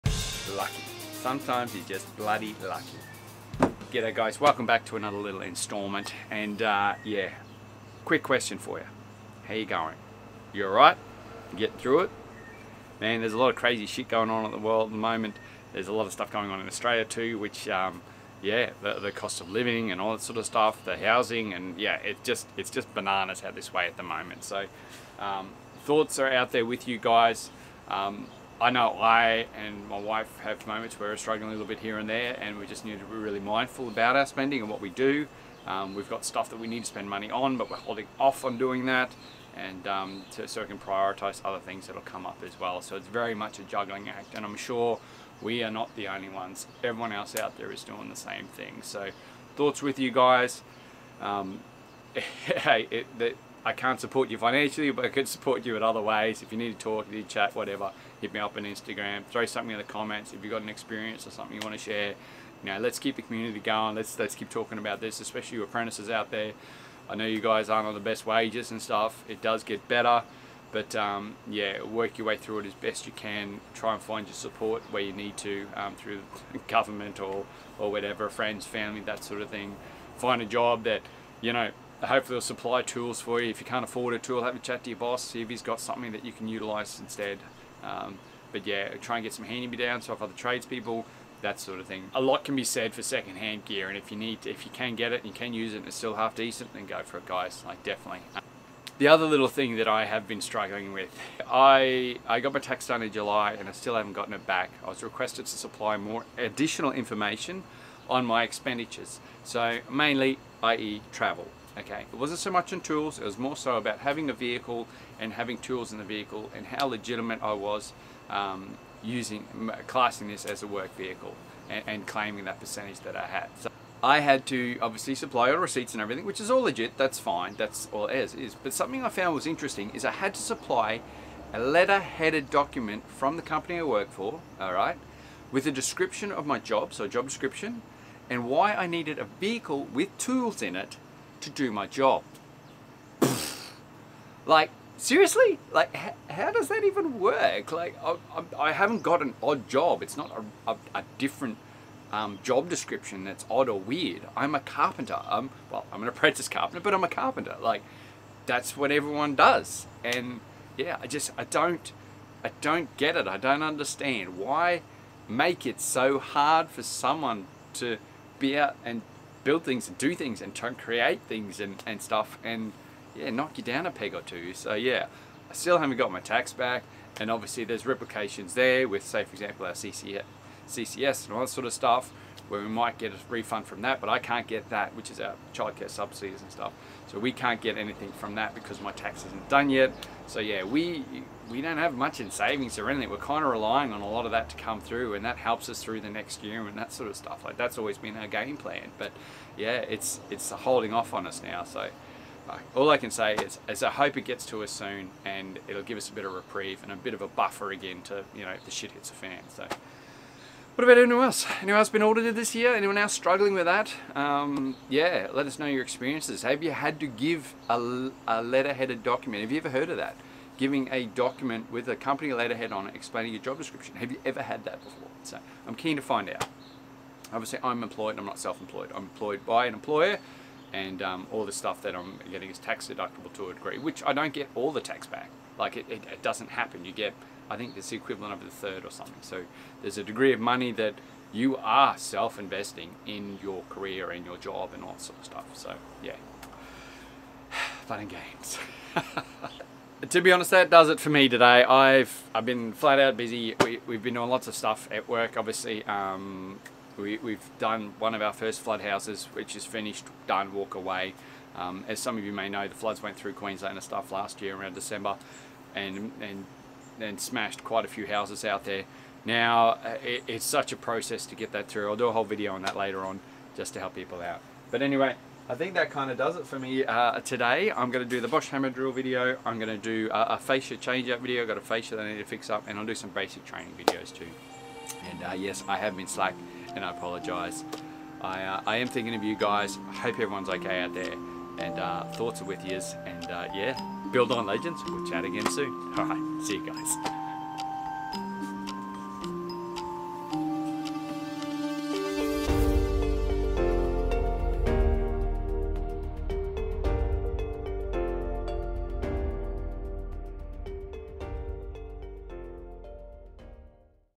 Things can be hard money wise as an Apprentice.
Come join as we have a quick chat and check in with you guys to see how everyone is going.